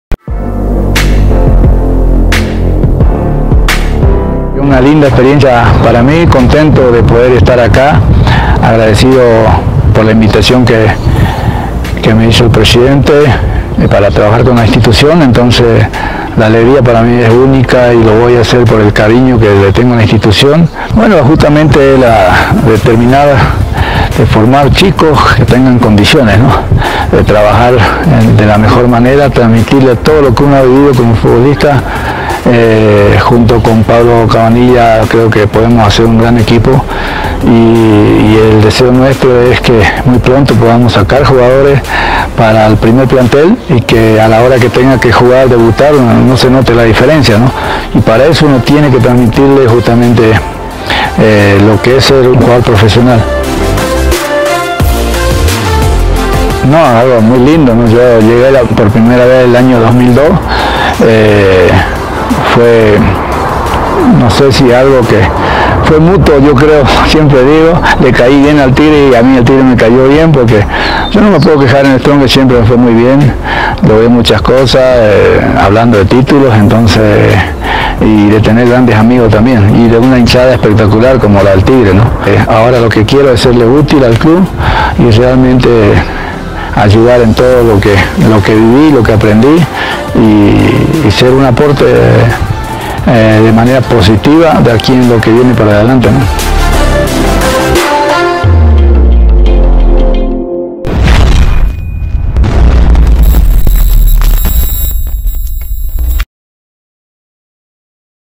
CRISTALDO TRABAJARA CON LAS DIVISIONES MENORES
“Estoy muy feliz de estar nuevamente acá, ahora en otra función pero con la misma alegría de cuando era jugador. Al ‘Tigre’ siempre le daré lo mejor y trabajaremos con los chicos para poder aportar al primer plantel. Es una felicidad volver a ponerme este buzo y sentir todo esto, tengo un club en Santa Cruz donde ya estamos tres años y hay jugadores que tranquilamente podrían jugar en la profesional entonces haremos un buen trabajo coordinado”, señaló Luis Héctor Cristaldo en conferencia de prensa. Más información en CompetenciaBolivia de lunes a viernes a las 13 y 19 hrs.